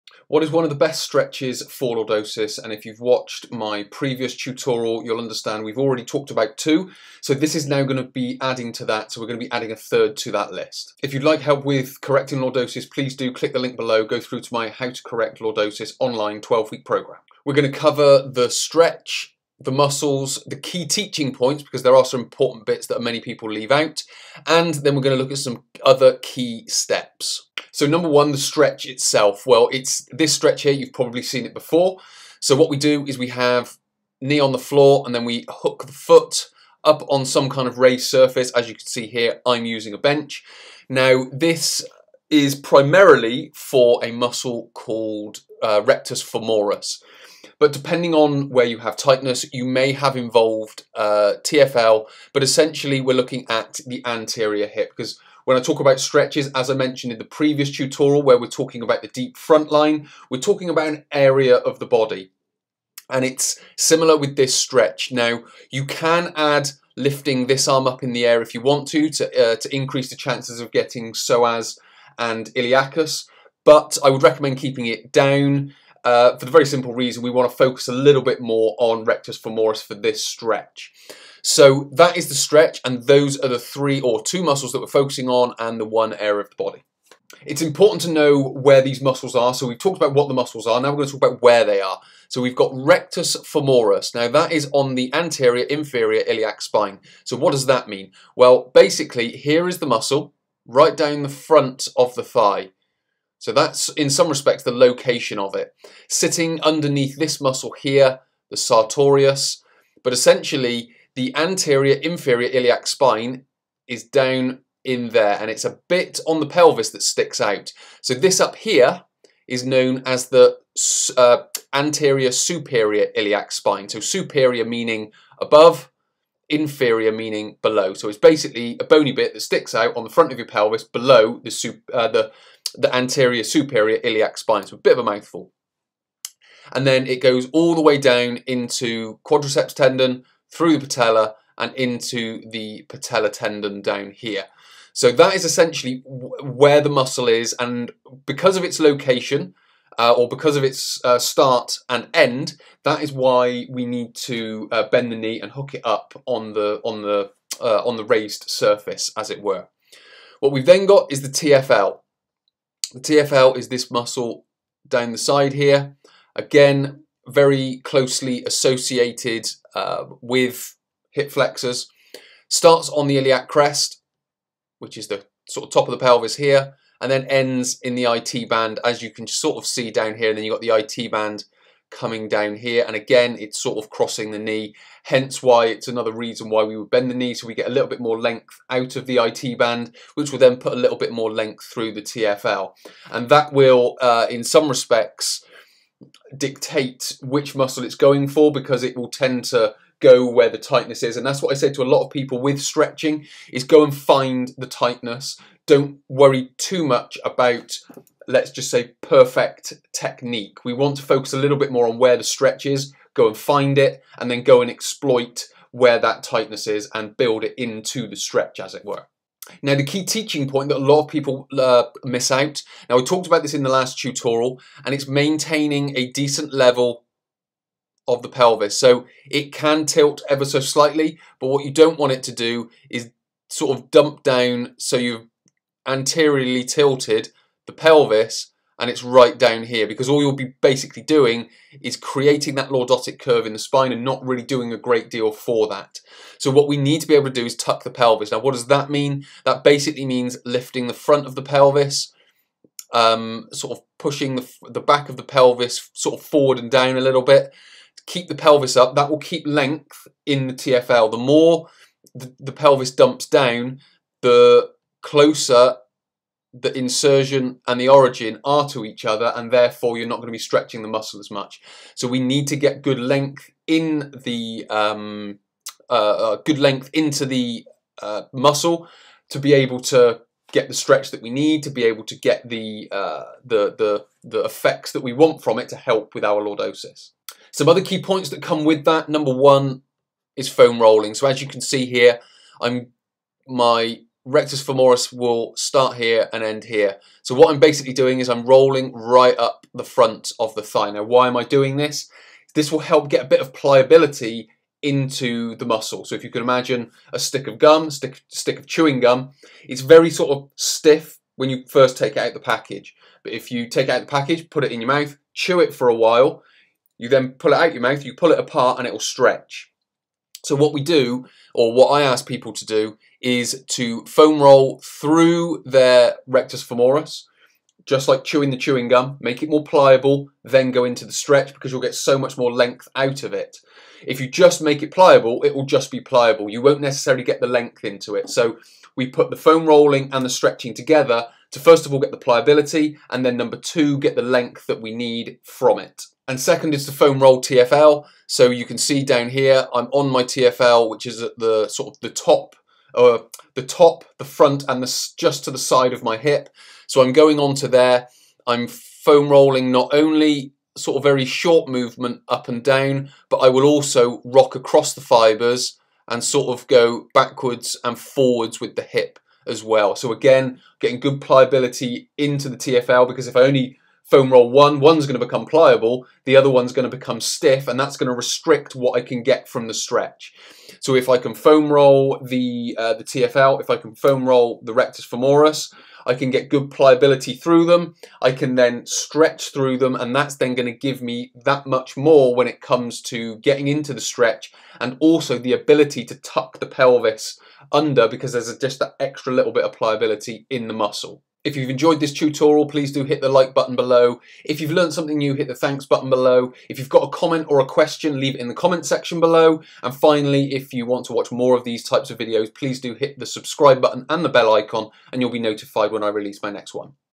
One of the best stretches for Lordosis
Do you have an excessive lumbar curve? If you want help reducing it there are number of stretches you can use. In this tutorial you'll learn one of the best stretches, the muscles it targets and key teaching points to include.